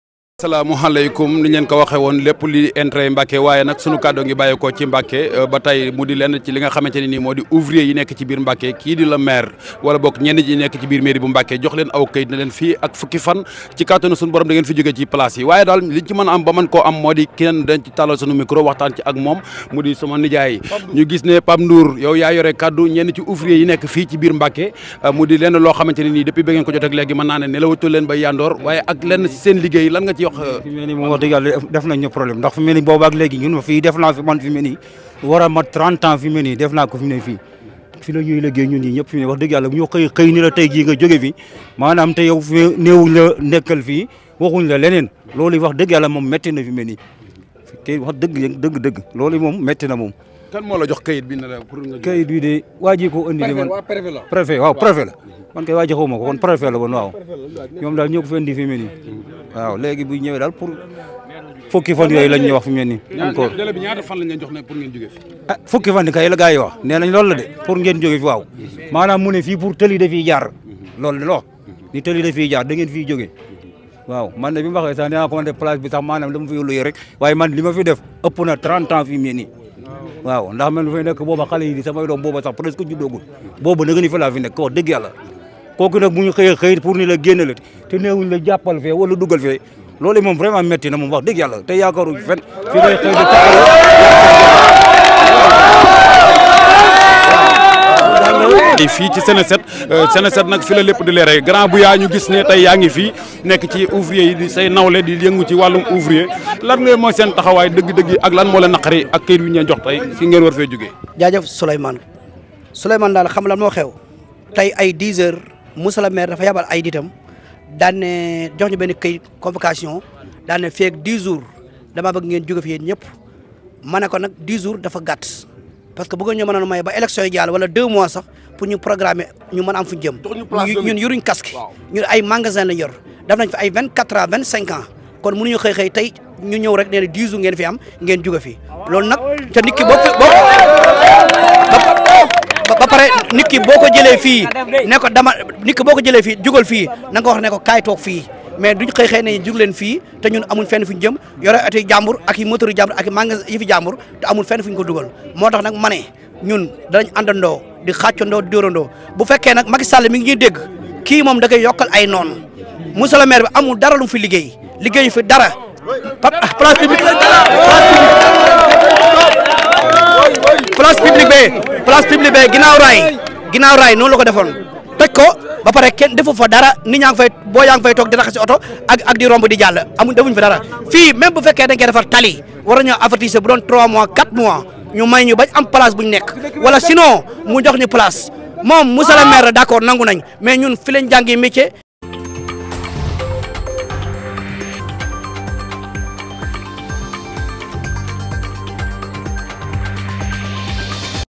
M'Backé -Les mécaniciens dans la rue pour contester les opérations de déguerpissement de la mairie
M'Backé -Les mécaniciens dans la rue pour contester les opérations de déguerpissement de la mairie prévis dans une dizaine de jours .Ils demandent un report pou leur laisser le temps de se préparer. Sénéjet est allé à leur rencontre .